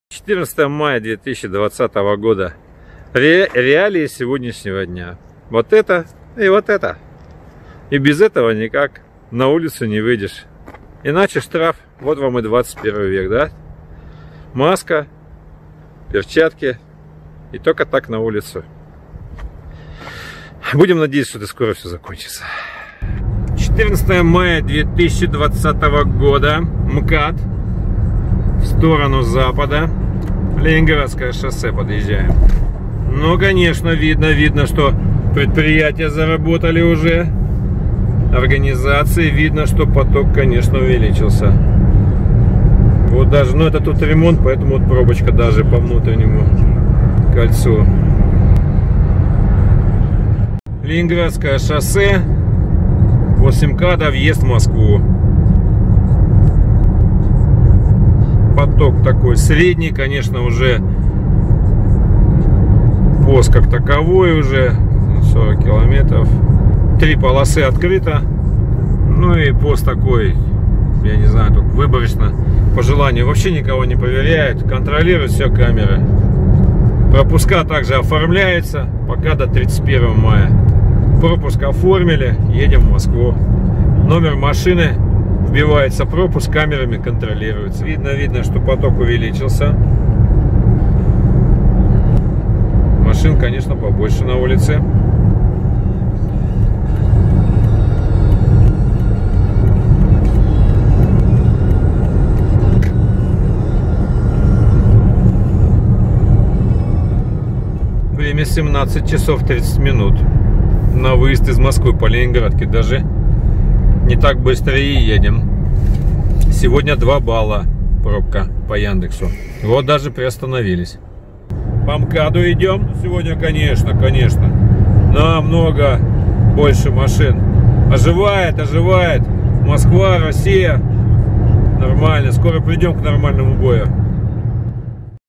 Поездка в Москву 14.05.2020 года. Трафик на дорогах Москвы.
Поездка по пропуску в Москву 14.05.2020 года. Трафик на дорогах Москвы.